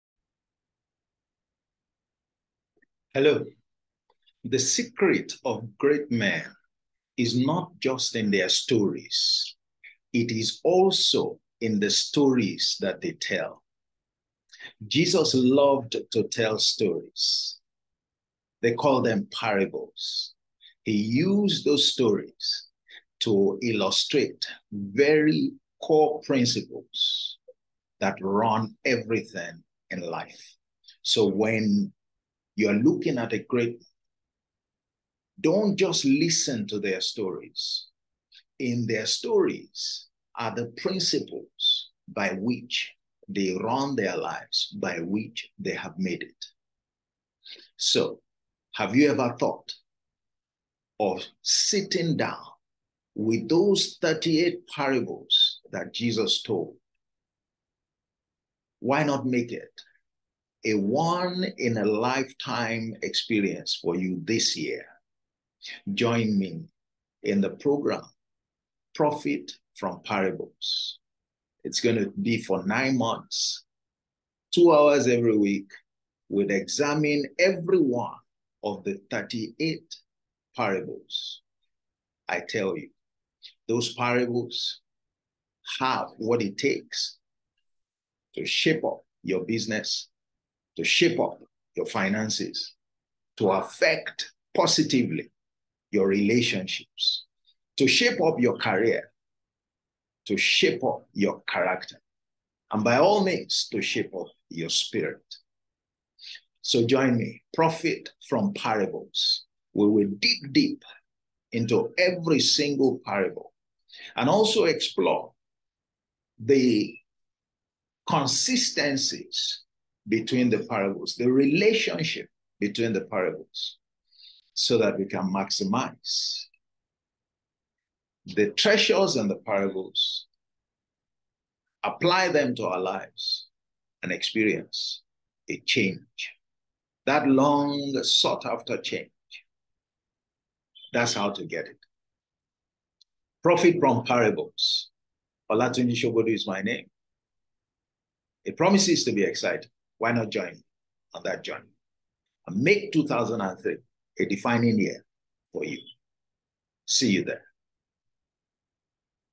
Profit From Parables Video Ad 1
Join the PROFIT FROM PARABLES (PFP) program.

Enrich your life through a journey of discovering the gems that successful people engage in their daily walk.

Those gems are hidden in the parables that Jesus told and a guided study of the same with an emphasis on how you can apply the principles to your life is a smart move.